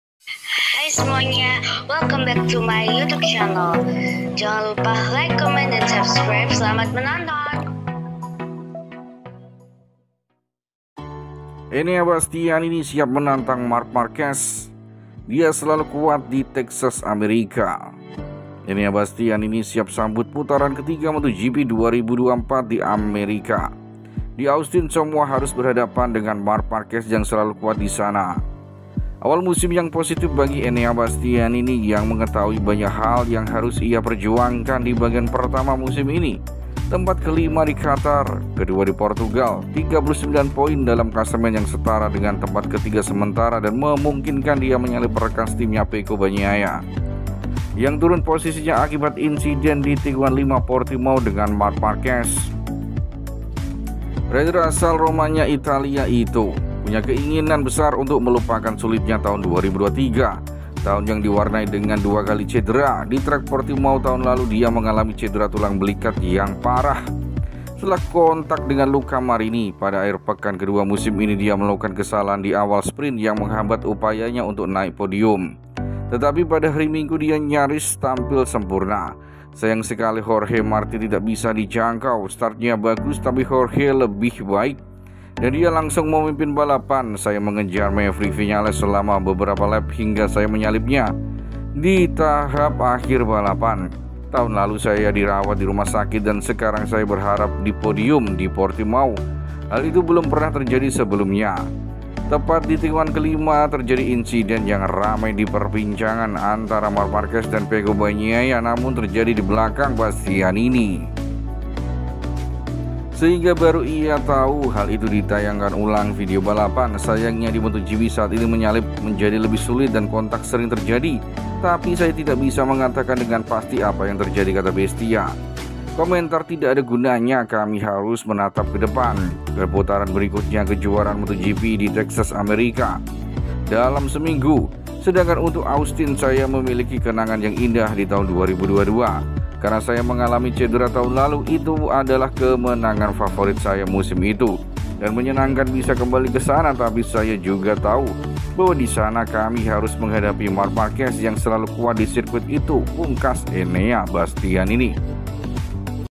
Gila! Bestia Pole Di MotoGP Amerika? Bastianini Siap Menantang Marquez: Dia Selalu Kuat di Texas!
Enea Bastianini siap sambut putaran ketiga MotoGP 2024 di Texas setelah naik podium di Portimao. Di Austin semua harus berhadapan dengan Marc Marquez yang selalu kuat disana.